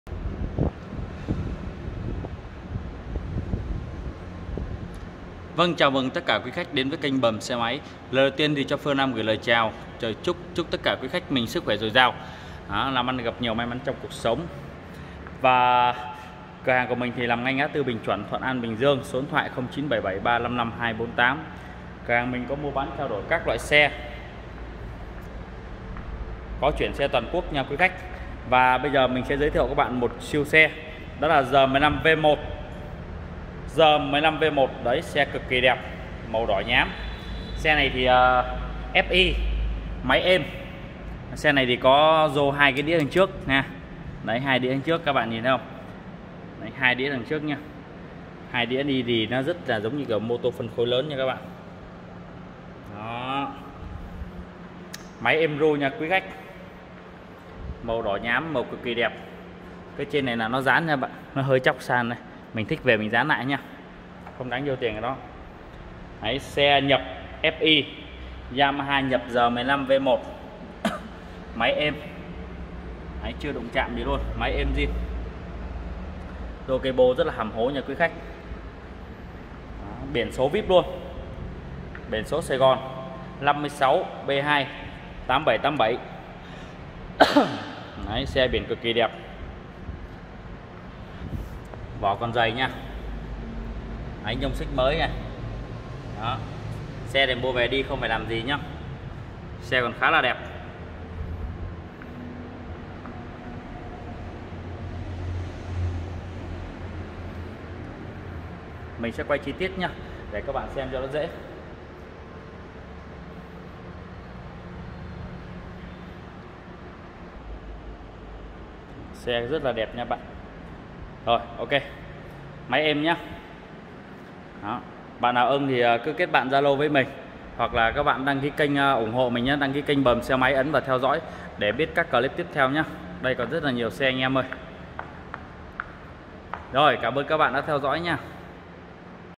Bờm xe máy# moto giá rẻ. thanh lý xe r15 v1 giá rẻ. đt 0977355248
cửa hàng cần thanh lý một chiếc R15 V1 xe đẹp máy em mọi chức năng hoạt động tốt có dùng hai cái đĩa ra trước địa chỉ ngã tư Bình Chuẩn Thuận An Bình Dương số điện thoại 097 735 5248 các bạn xem kênh nhớ đăng ký và ấn chung theo dõi nhé